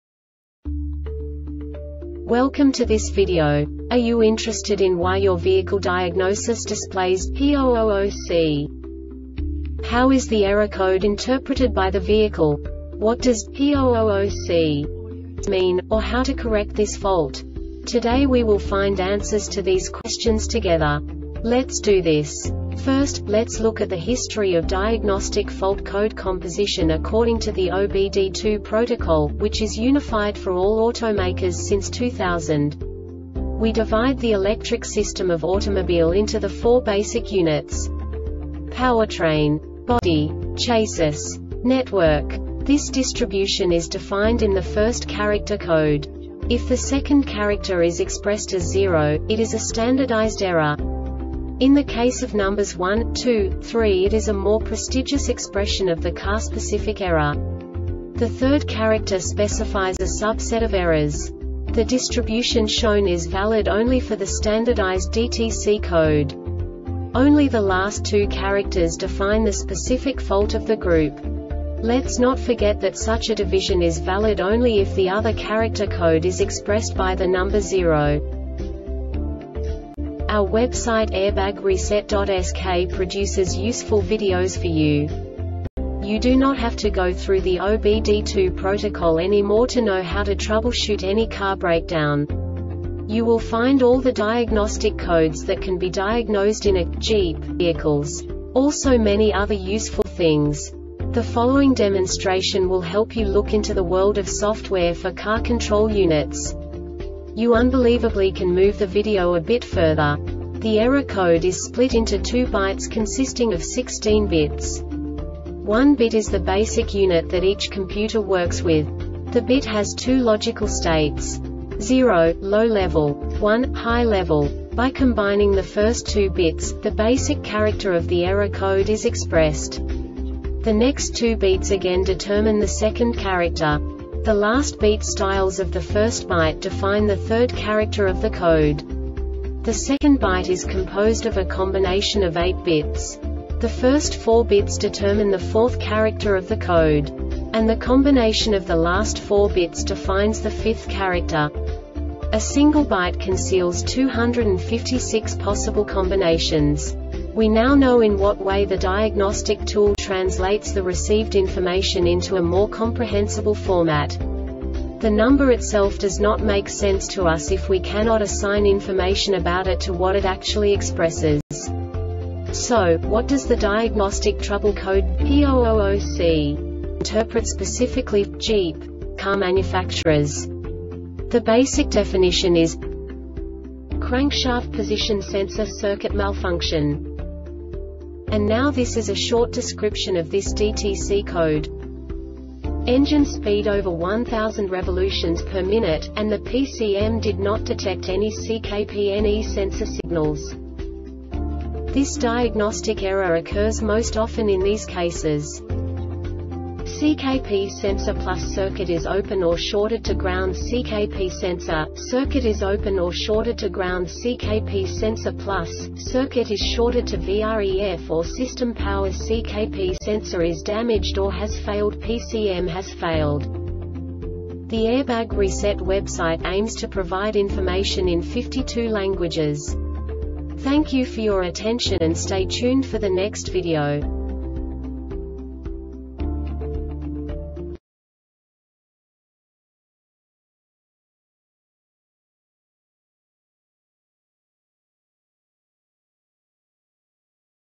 DTC Jeep P000C Short Explanation
Contents:
0:21 Basic DTC analysis according to OBD2 protocol standard.
1:48 Insight into programming
2:58 A diagnostically understandable description of the Diagnostic Trouble Code
3:05 Basic error definition

Make
Jeep
Code
P000C
Definition
Crankshaft Position Sensor Circuit Malfunction
Description
Engine speed over 1000 rpm,  and the PCM did not detect any CKP NE sensor signals
Cause
CKP sensor + circuit is open or shorted to groundCKP sensor - circuit is open or shorted to groundCKP sensor +,  - circuit is shorted to VREF or system powerCKP sensor is damaged or has failedPCM has failed